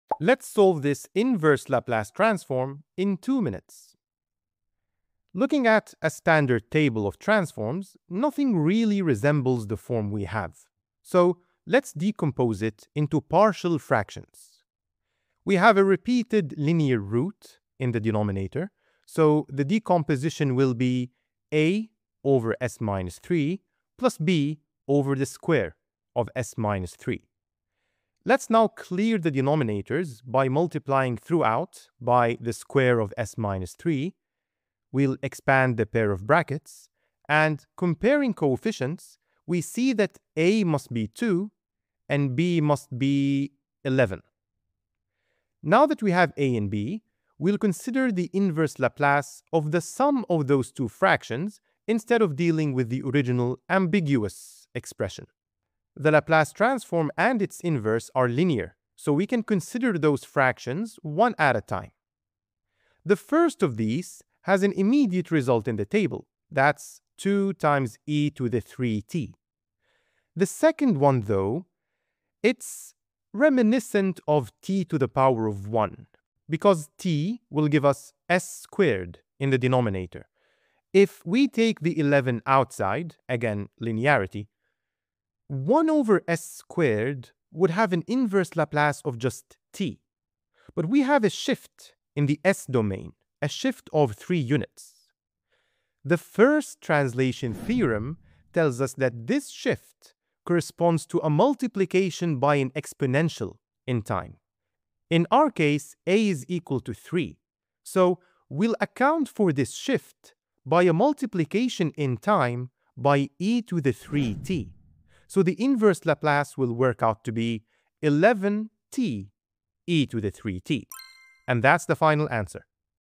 Inverse Laplace Transform Using Partial Fractions
Let’s find the inverse Laplace transform of this expression in 2 minutes.

We’ll decompose it into partial fractions and then use the First Translation Theorem of the Laplace transform.